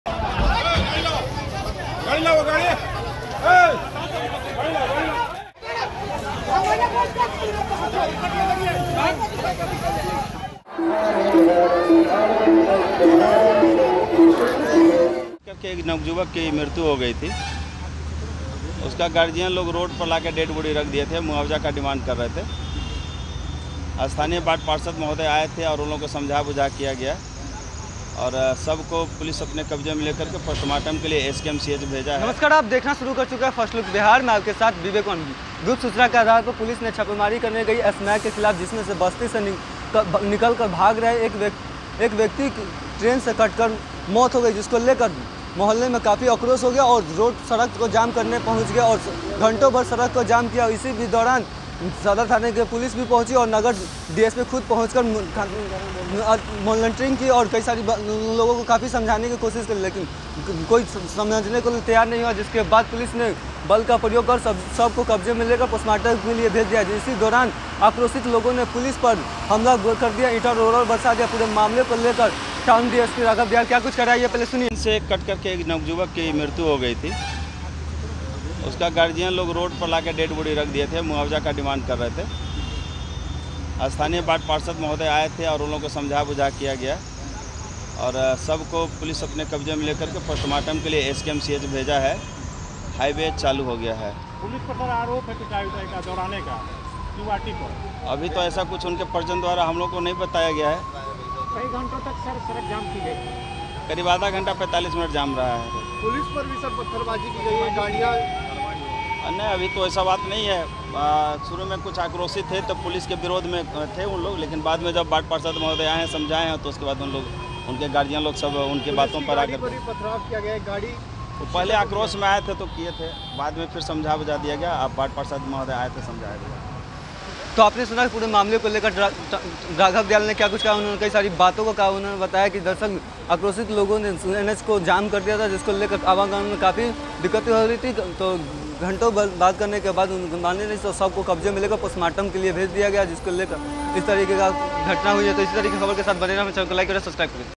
Police की छापेमारी से भाग रहे एक व्यक्ति की Train से कटने पर आक्रोश, किया तोड़फोड़, सड़क जाम
Police की छापेमारी से भाग रहे एक व्यक्ति की Train से कटने पर आक्रोश, Police की गाड़ी की का तोड़ा शीशा, सड़क जाम | Bihar | First Look Bihar |

Please like our Facebook page - First Look Bihar